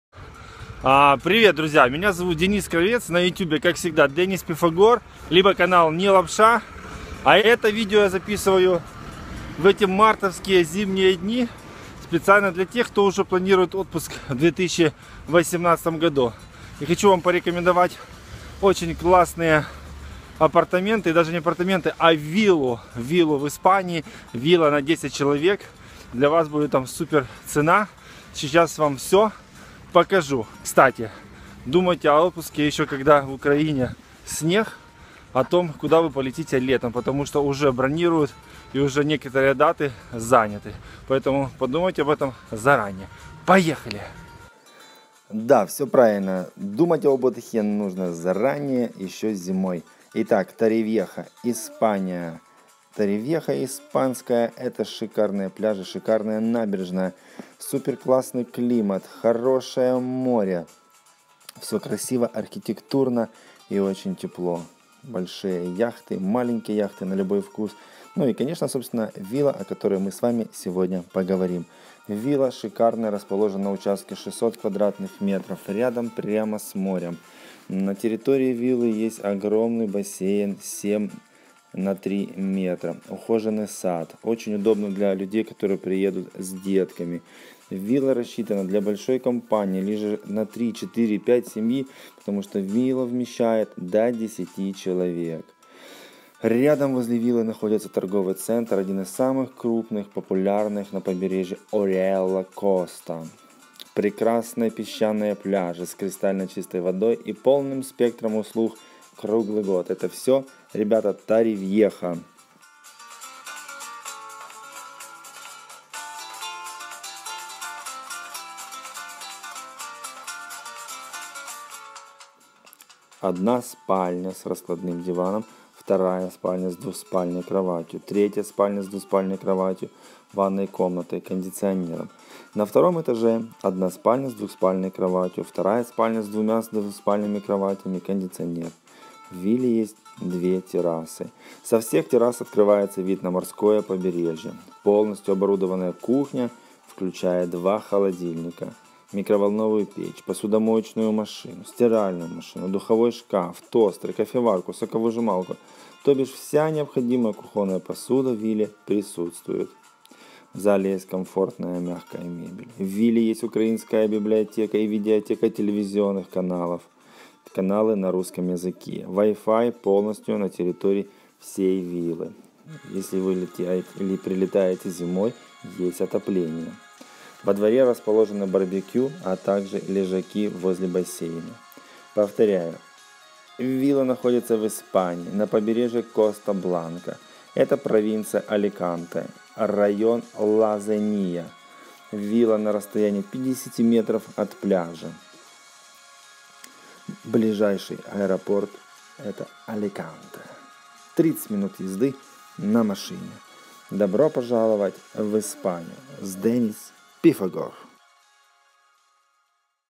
Аренда виллы в Испании на 10 чел.
Шикарная вилла, расположенная на участке 600кв.м.
- Большой бассейн 7х3 м;
-  ухоженный сад;
- Рядом находится торговый центр, один из самых популярных на побережье Ориуэла коста;
-  Прекрасные песчаные пляжи с кристально чистой водой и полным спектром услуг круглый год;
-  Рядом расположен новый коммерческий центр Zenia Boulevar с многочисленными бутиками, магазинами, супермаркетом, кафе;
- Гольф поле в 4 км, Аквапарк в 7км.;
-  Протяжённый песчаный пляж La Zenia всего в 50 метрах, предлагающий занятия многочисленными водными видами спорта, а так же не менее популярные пляжи Punta Prima, Cabo Roig, Playa Flamenca в 5-10 минутах езды.
-  Вилла в два уровня включает 5 спальных комнат. Идеально подходит для 3-5 семей или большой компании;
- На первом этаже расположены: зал с большим диваном, камином, ванной комнатой, установлен кондиционер, с выходом на террасу. Одна спальня с раскладным диваном. Вторая спальня с двухспальной кроватью. Третья спальня с двуспальной кроватью, ванная комната, кондиционер.
-  На втором этаже расположены: Одна спальня с двуспальной кроватью. Вторая спальня с двумя односпальными кроватями;
-  кондиционер;
- две террасы. Со всех террас открывается вид на морское побережье.
- Полностью оборудованная кухня, включает два холодильника, микроволн. печь, посудомоечная и стиральная машины, духовой шкаф, тостер, кофеварка, соковыжималка, вся необходимая кухонная посуда;
-  В зале комфортная мягкая мебель.
- В вилле есть украинская библиотека и видеотека, ТВ на русском языке, WI-FI, отопление;
-  Во дворе расположен барбекю, а так же лежаки;
       Побережье: Коста Бланка
Провинция: Аликанте
Район: Ла Зения
Пляж в 50 метрах.
стоимость 2000 евро за неделю за 10 человек.
В летний сезон 2500 евро за 10 человек.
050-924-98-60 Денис